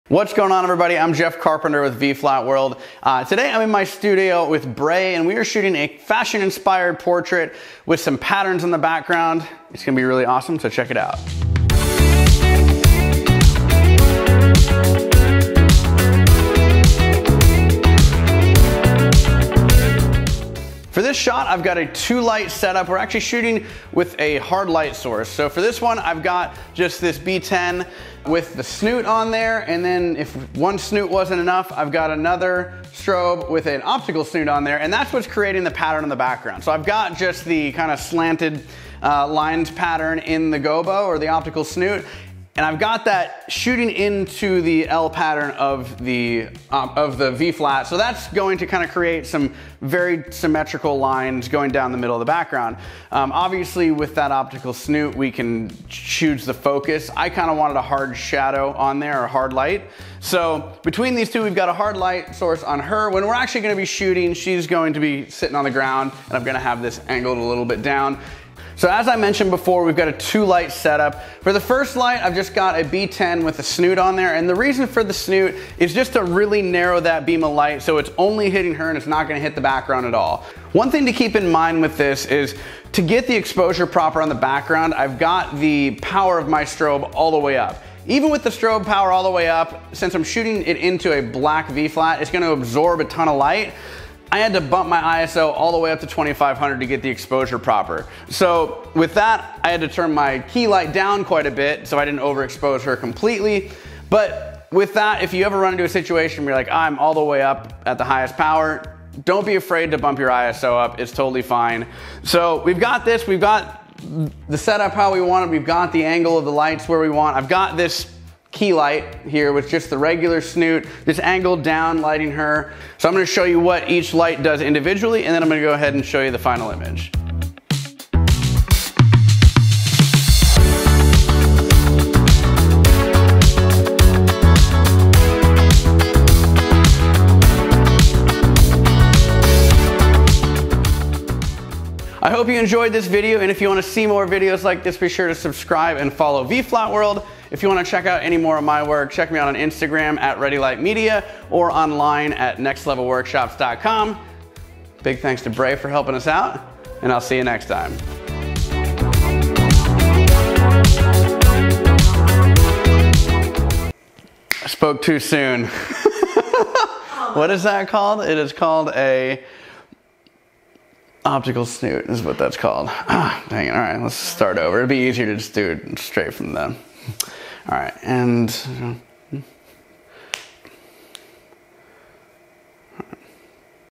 Light it Like Jeff: Creating Light Patterns On A Background | Ep 18
Jeff Carpenter shows us here how to create a chevron pattern lighting effect on the background, which is the black side our the V-Flat.